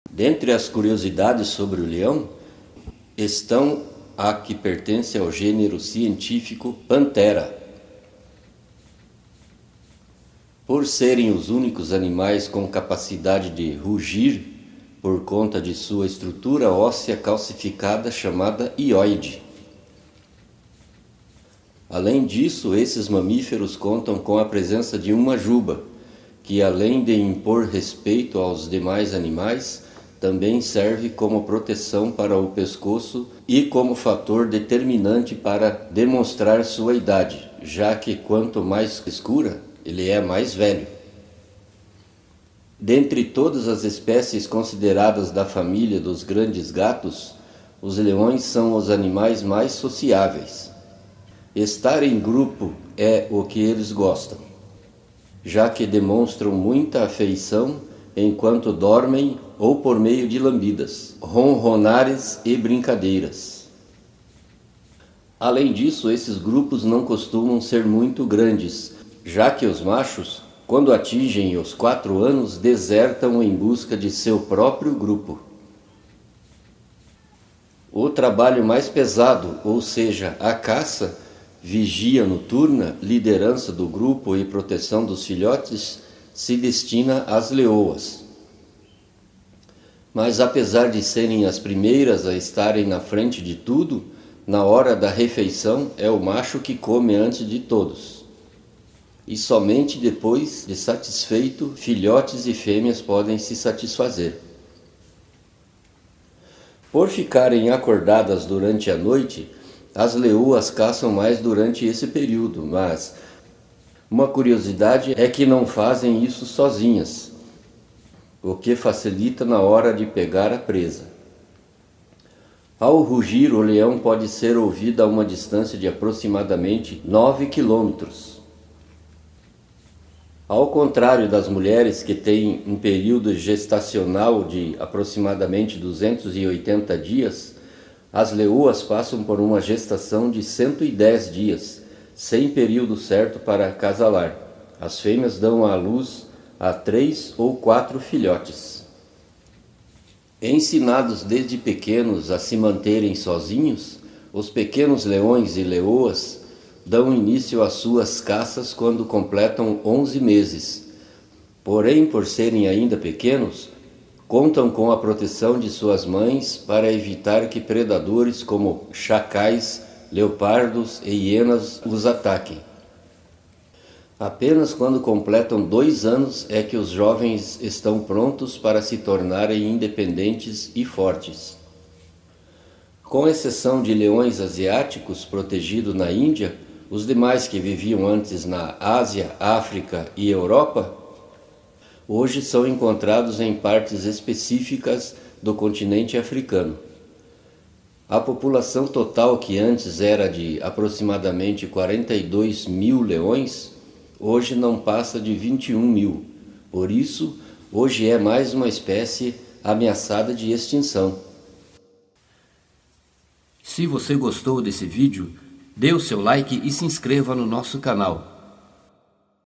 Fatos CURIOSOS sobre os LEÕES | Natureza Viva
Dentre todas as espécies consideradas da família dos grandes gatos, os leões são os animais mais sociáveis. Estar em grupo é o que eles gostam, já que demonstram muita afeição enquanto dormem ou por meio de lambidas, ronronares e brincadeiras.

Seja bem vindo (a) ao canal NaTuReZa ViVa!